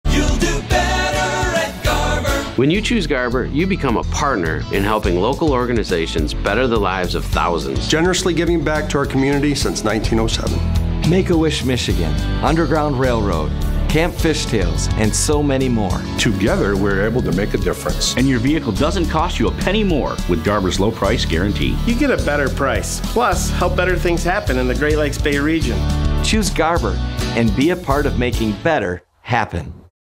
Garber Automotive Group - Super Bowl 2023 Commercial
Super Bowl 2023 commercial for  Garber Automotive Group, created for Houser and Hennessee advertising by  ProdNation LLC  @noiseblender @TerryPhillipsVO

Video Production, editing, motion graphics, sound design, podcast production, and more cut through the noise with NoiseBlender.